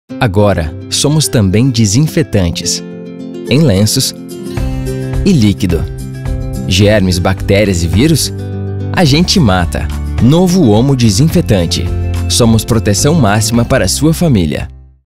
Agora sOMOs também desinfetantes
Novo OMO Desinfetante, o Exterminador de Bactérias, disponível em lenços e líquidos.

Germes, bactérias e vírus? 🦠 A gente mata! 😎

sOMOs proteção máxima para a sua família. 💙